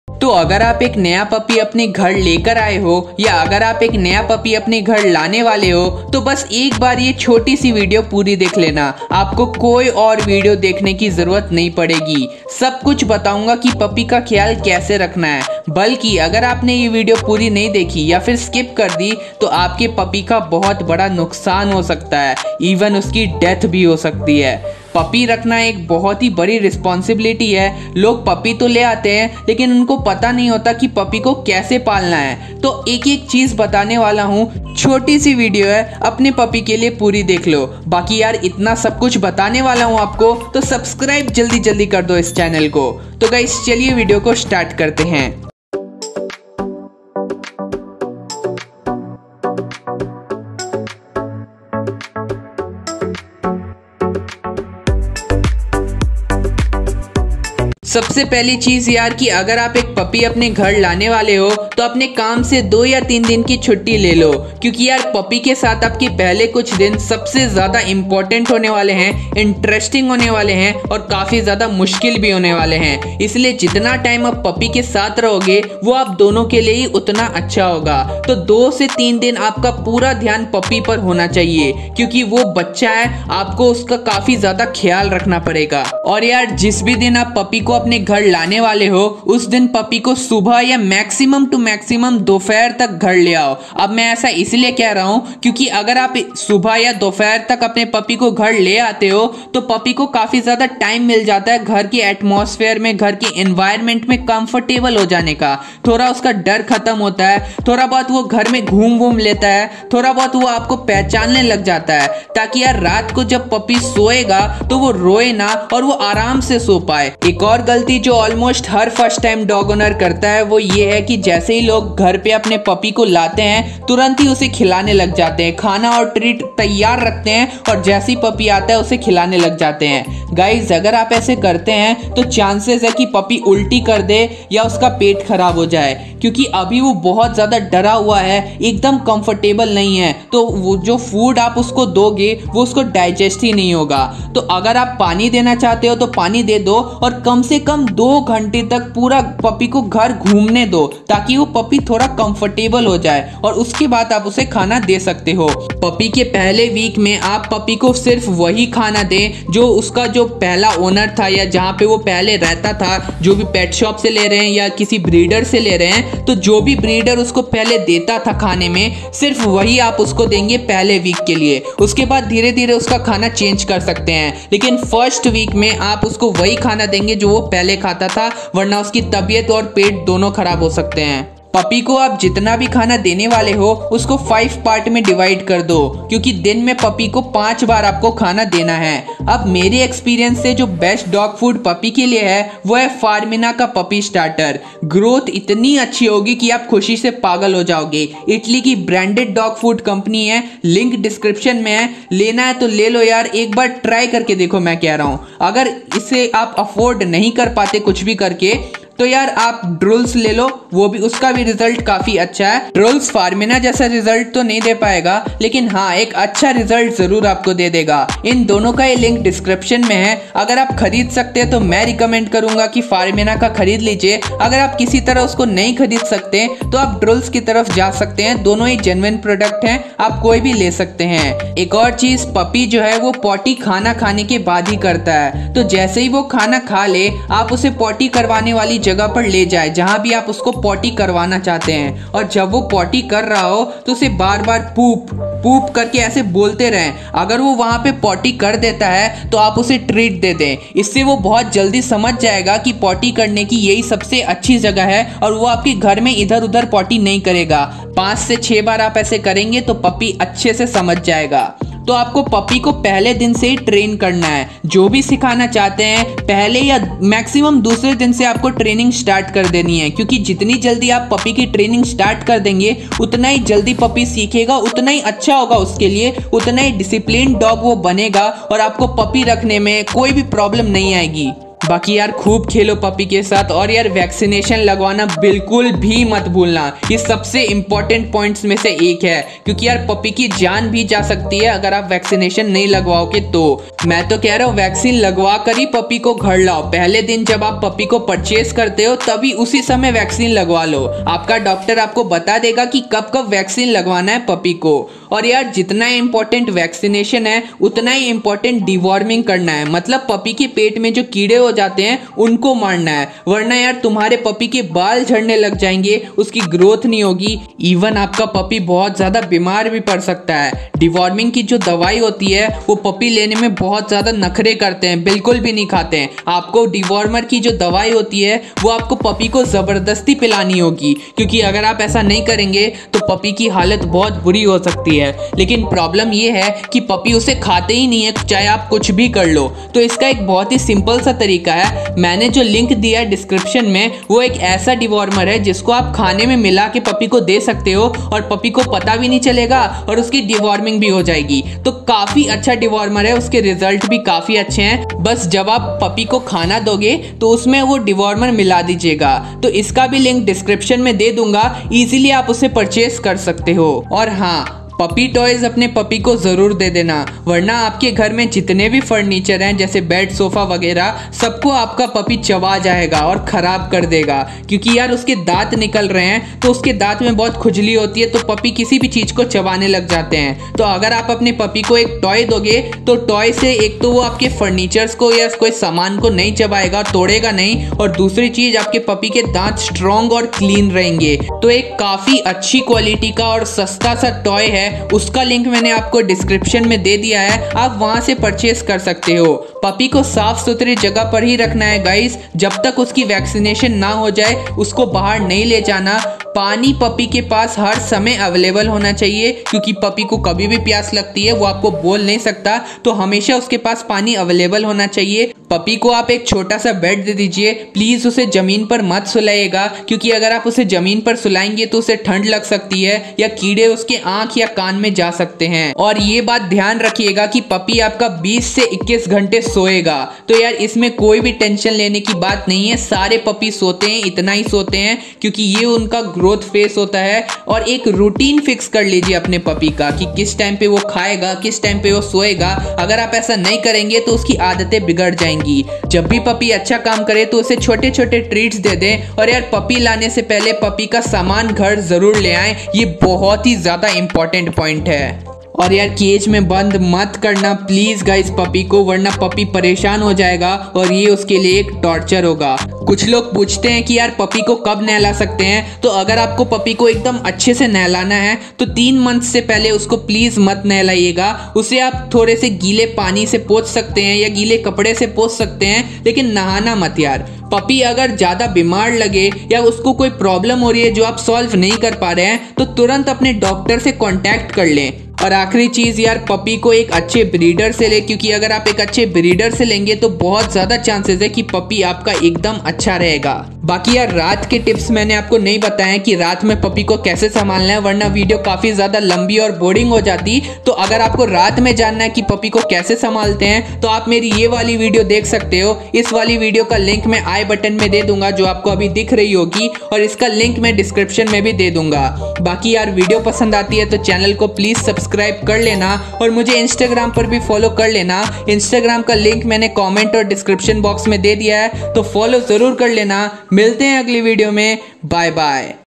Full Guide : How To Take Care of New Puppy in 1st Week in Hindi
Hey Guys in this video I am going to tell you how to take care of new puppy in first week or you can say how to take care of new puppy at home easily.. Basically you will learn how to handle 1 month old new puppy without it's mother. .. How to take care of puppy of any breed like golden retriever, Labrador, german Shepherd, Siberian husky or small breeds like beagle, pug, Pomeranian.. I will tell you about Care of a New Puppy: The Basic Rules That You Must Follow...

1) BEST FOOD FOR YOUR PUPPY ;

A) FARMINA PUPPY STARTER(2.5kg)

B) DROOLS PUPPY STARTER (10kg)

2) BEST DEWORMER FOR PUPPY

3) BEST TOYS FOR PUPPY